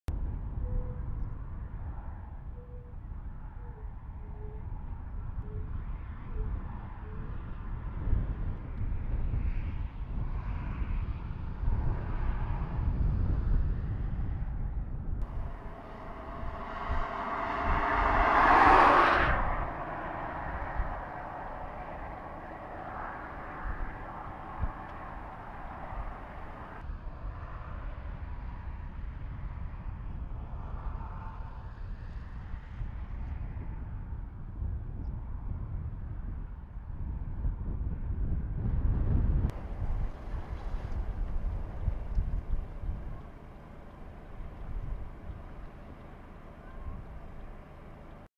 BYRON/ White Trumpeter Swans
Thank you to Linda DeBellis Johnson for the heads up on the White Trumpeter Swans hanging out on Bank street road and Cockram road today